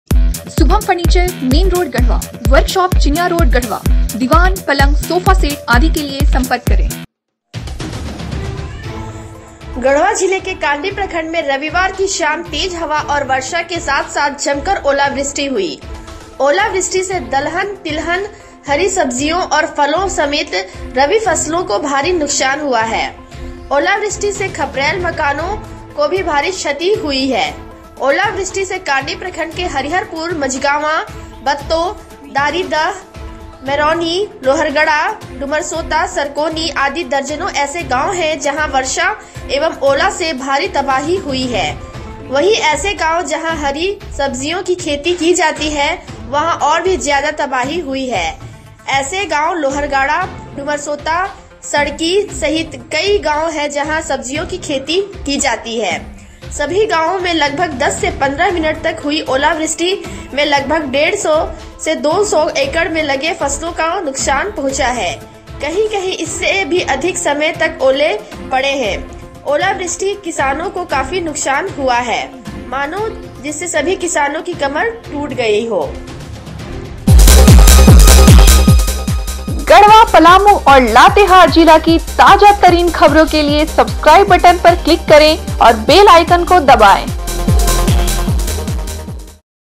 कांडी। गढ़वा। आसमान ने उगले ओले , किसानों की टुटी कमर
कांडी। गढ़वा। गढ़वा जिले के कांडी प्रखंड में रविवार की शाम तेज हवा और वर्षा के साथ साथ जमकर ओलावृष्टि हुई। ओलावृष्टि से दलहन, तिलहन, हरी सब्जियों और फलों समेत रबी फसलों को भारी नुकसान हुआ है। ओलावृष्टि से खपरैल मकानों को भी भारी क्षति हुई है। ओलावृष्टि से कांडी प्रखंड के हरिहरपुर, मझिगावां, बत्तों, दारीदह, मेरौनी, लोहरगाड़ा, डुमरसोता,सरकोनी आदि दर्जनों ऐसे गांव है जंहा वर्षा एवं ओला से भारी तबाही हुई है।वंही ऐसे गांव जंहा हरि सब्जियों की खेती की जाती है वंहा और भी ज्यादा तबाही हुई है ऐसे गांव लोहरगाड़ा, डुमरसोता, सड़की सहित कई गांव हैं जंहा सब्जियों की खेती की जाती है।
 सभी गांवो में लगभग दस से पंद्रह मिनट तक हुई ओलावृष्टि में लगभग डेढ़ से दो सौ एकड़ में लगे फसलों को नुकसान पंहुचा है।कहीं कहीं इससे भी अधिक समय तक ओले पड़े हैं। ओलावृष्टि किसानों को काफी नुकसान हुआ है। मानो जिससे सभी किसानों की कमर टूट गए हों।Talash TV
Latest News
Breaking News
Current News
garhwa news
palamu news
latehar news